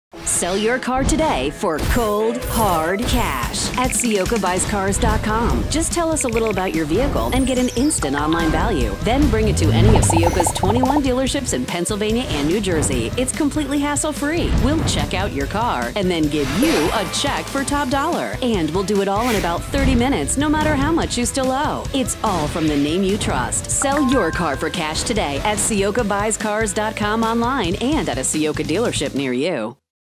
Ciocca Buys Cars
Looking to sell your vehicle? Ciocca can help with that too! Our network of Ciocca Buys Cars locations across Pennsylvania and New Jersey makes it easy for you to get fast cash for your car.